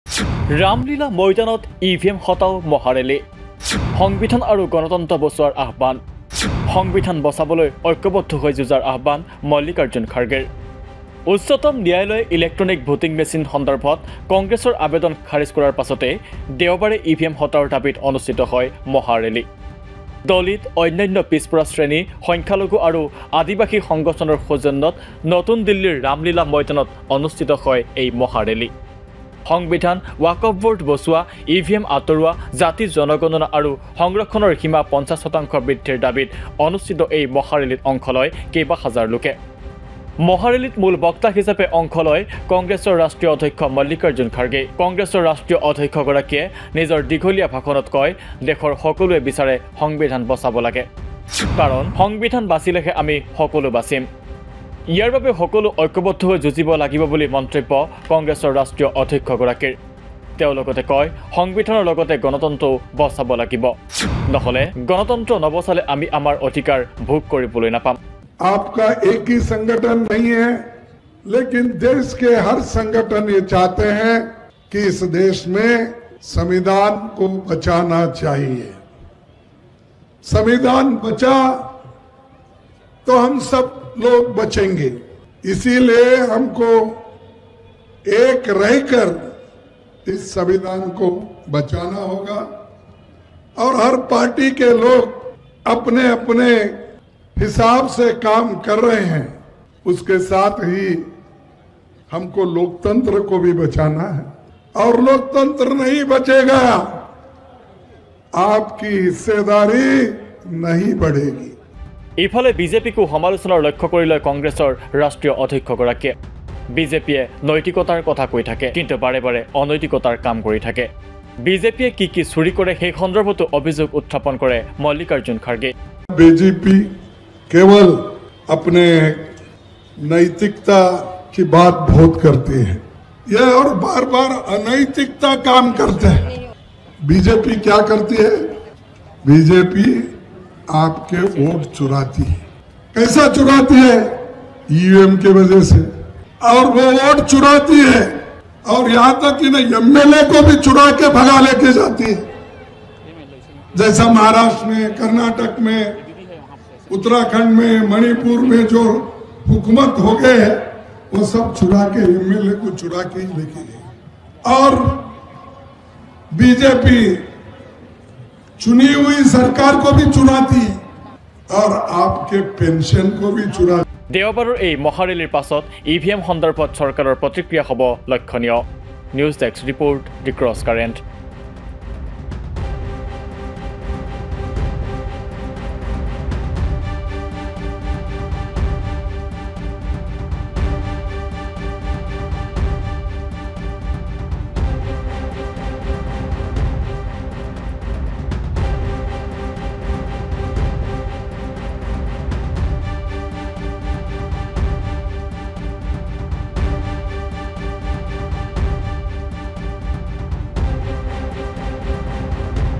ই ভি এম হটাওৰ দাবীত মহাৰেলী
ই ভি এম হটাওৰ দাবীত মহাৰেলী। ৰামলীলা ময়দানত হাজাৰ হাজাৰ লোকৰ সমাৱেশ। ৰামলীলা ময়দানত ই ভি এম হটাও মহাৰেলী। সংবিধান আৰু গণতন্ত্ৰ বচোৱাৰ আহ্বান। সংবিধান বচাবলৈ ঐক্যবদ্ধ হৈ যুঁজাৰ আহ্বান মল্লিকাৰ্জুন খাৰ্গেৰ।The Crosscurrent is an independent newsroom that produces investigative and analytical reporting with dedication.